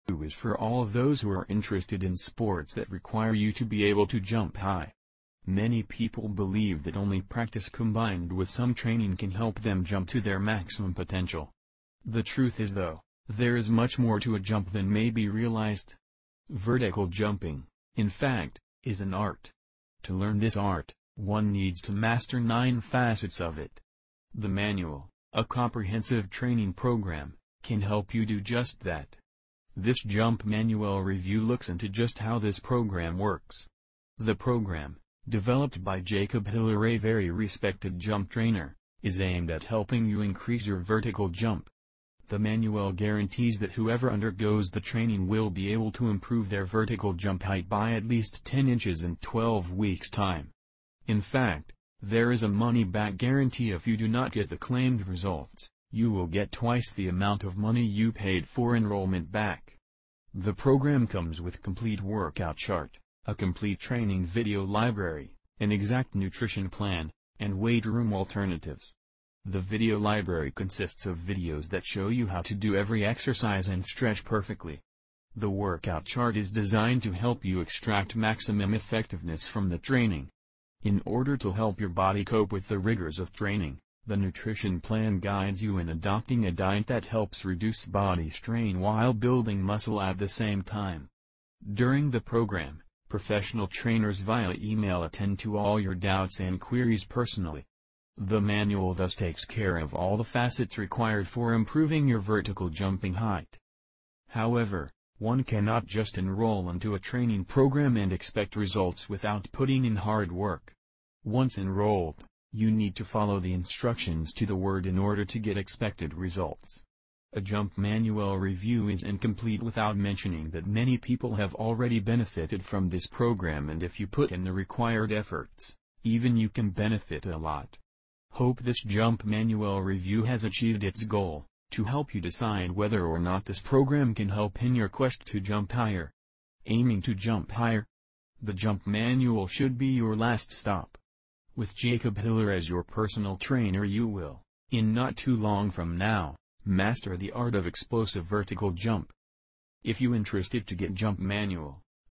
Watch This Jump Manual Review First Before You Buy Or Waste Your Money On Useless Things.
- Jump manual review. This jump manual review is for all those who are interested in sports that require you to be able to jump high. Many people believe that only practice combined with some training can help them jump to their maximum potential. The truth is though, there is much more to a jump than may be realized. Vertical jumping, in fact, is an art. To learn this art, one needs to master nine facets of it. The manual, a comprehensive training program, can help you do just that. This jump manual review looks into just how this program works.

The program, developed by Jacob Hiller-a very respected jump trainer, is aimed at helping you increase your vertical jump. The manual guarantees that whoever undergoes the training will be able to improve their vertical jump height by at least 10 inches in twelve weeks time. In fact, there is a money back guarantee-if you do not get the claimed results, you will get twice the amount of money you paid for enrollment back.

The program comes with complete workout chart, a complete training video library, an exact nutrition plan, and weight room alternatives. The video library consists of videos that show you how to do every exercise and stretch perfectly. The workout chart is designed to help you extract maximum effectiveness from the training. In order to help your body cope with the rigors of training, the nutrition plan guides you in adopting a diet that helps reduce body strain while building muscle at the same time. During the program, professional trainers via email attend to all your doubts and queries personally.